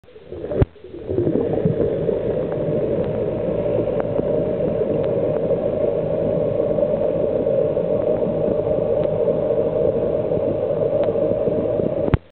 Asian Clam Bed Fly Over
UC Davis Tahoe Environmental Research Center (TERC) researchers study the Asian Clam in Lake Tahoe.